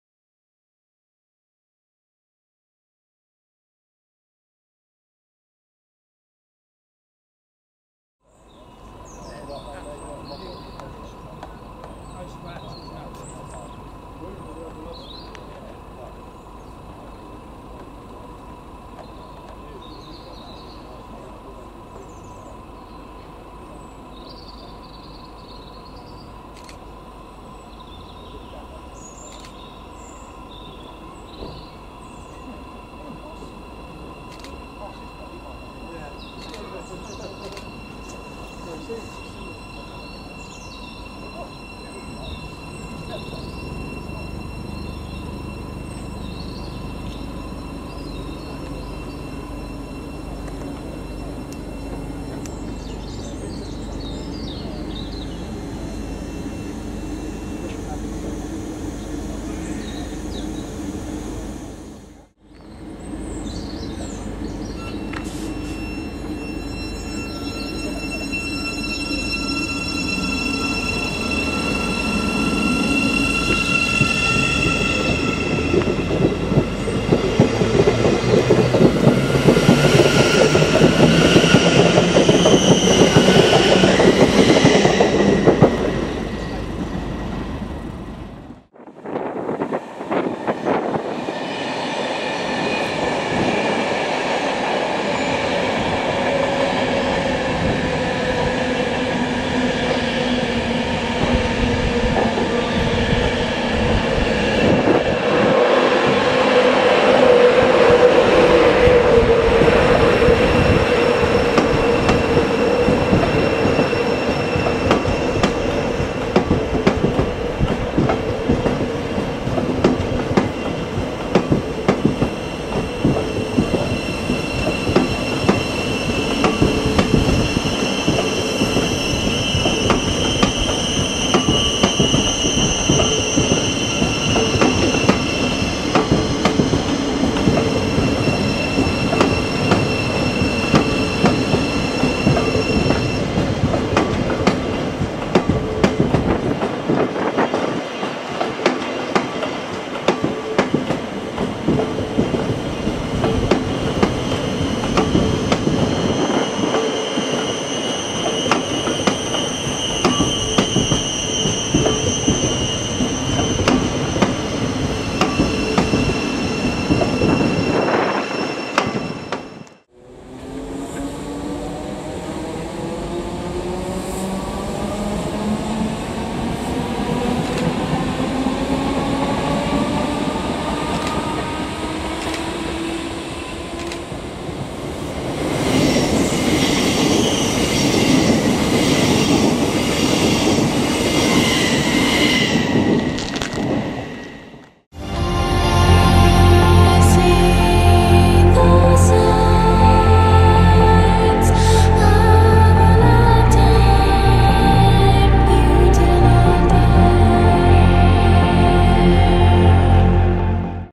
GN Class 717s at Bowes Park & Harringay. 31/10/18
Filmed: 31/10/18
Here we see Class 717007 at Bowes Park on a training run to Gordon Hill. Then we see Class 717020 & 018 at Harringay after being deivered earlier in the day and shortly after we see 717007 returning to Moorgate.
**I DON'T Sub for Sub so don't bother asking and don't advertise your own videos on my videos**